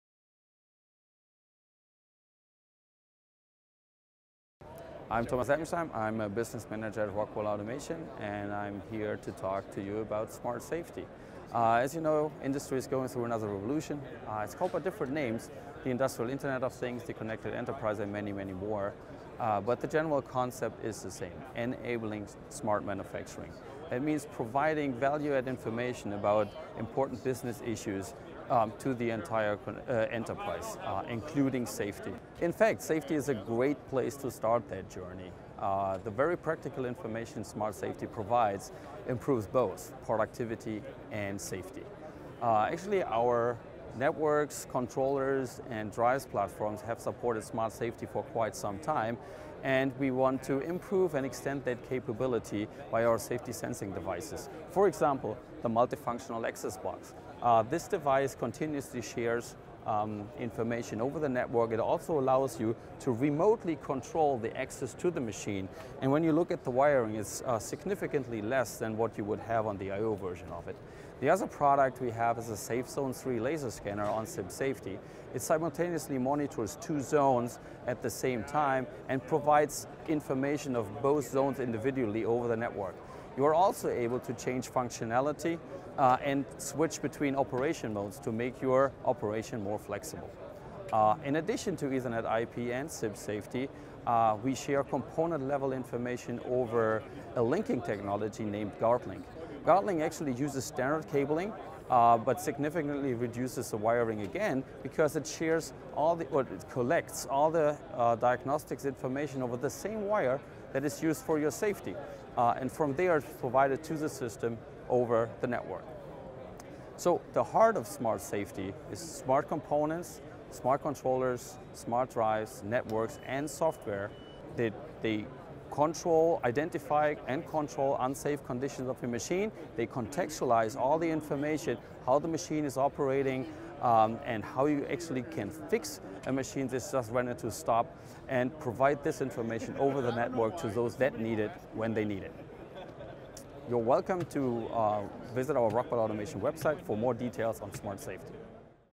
Enabling Smart Safety to Improve Performance
Identifying and controlling potentially unsafe conditions, contextualizing information based on machine condition, mode and expected results – and providing that insight where and when it’s needed is at the heart of integrated smart safety. Take a look at safety sensing devices that extend and improve smart safety – and complement Rockwell Automation networks, and Logix and drives platforms. Featured capabilities include GuardLink™ technology and the new Allen-Bradley® SafeZone™3 CIP safety laser scanner. Thomas Helpenstein, Rockwell Automation, provides details.